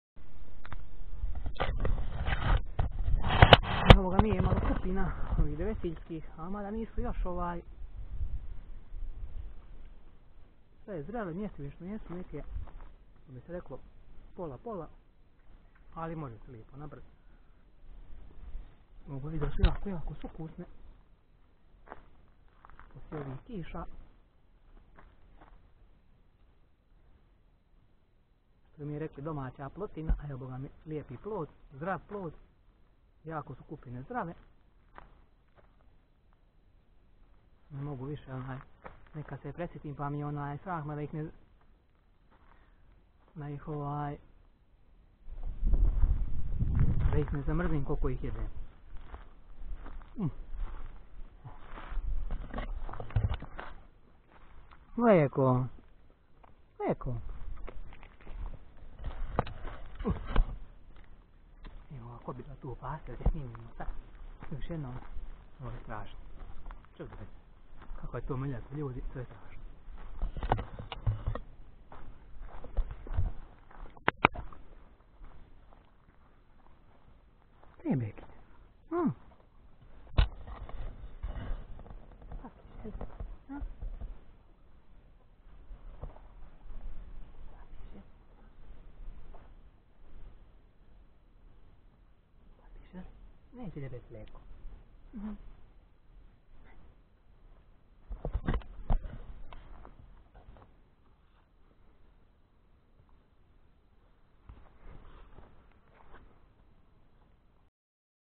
24H u sumi,nocenje,nadogradnja zaklona,zalazak-izlazak sunca i trekking ture 03-04. septembar 2021.
U povratku sa Sokolove Glave malo cupnuo kupina pored Rujevske Lokve i mazio konjica 👊💪👍